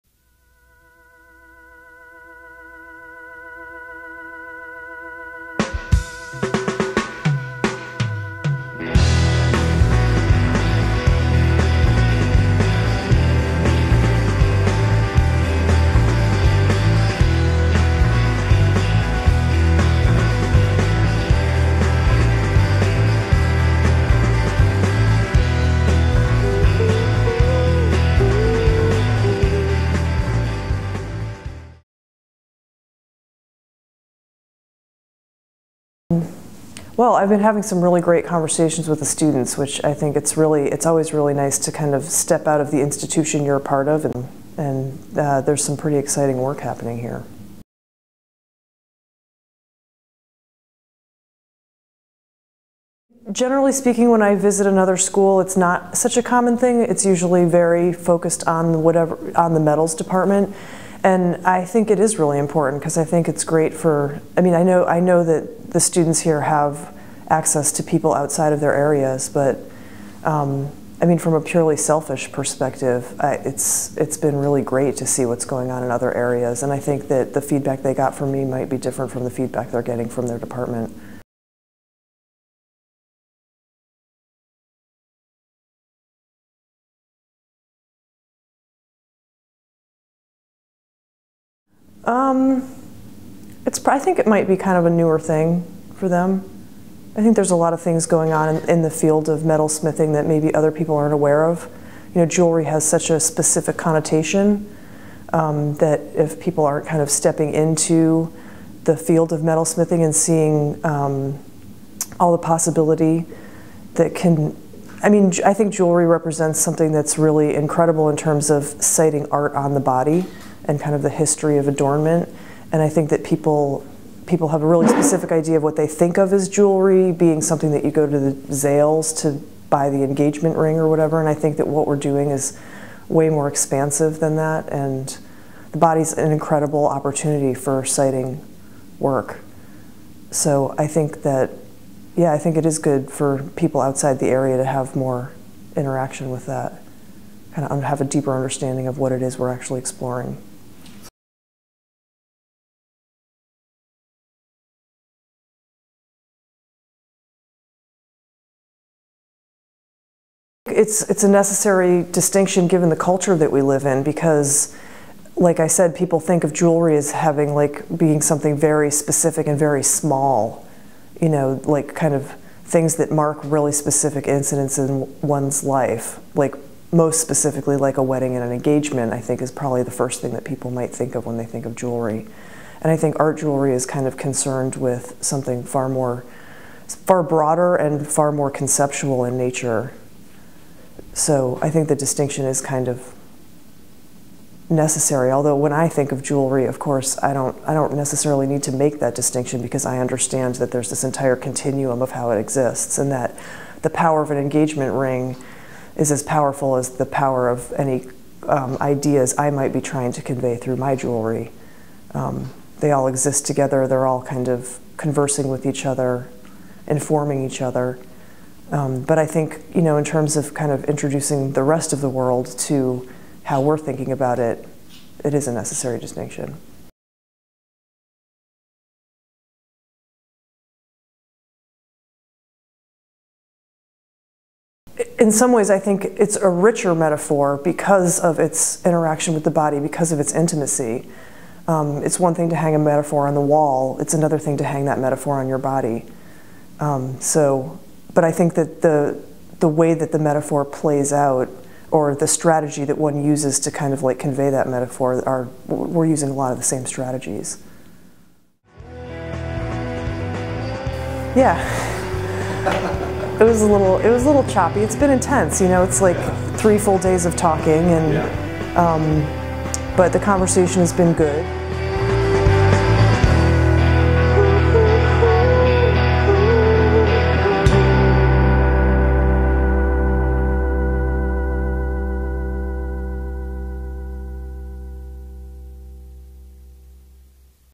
Lola Brooks LDSOA
2012-13 Dodd Professor Lola Brooks spent a few days at the Lamar Dodd School of Art in 2010 as a Visiting Artist. In the midst of that whirlwind visit, with a lecture and many students critiques, she was kind enough to sit down and talk with me about jewelry as an art form, as her art form, and the importance for students of talking with and to artists from a variety of disciplines. Great stuff, terrific glasses.

Video by Alan Flurry for LDSOA and the Franklin College of Arts and Sciences.

Theme by Athens, Ga band, SNAIL.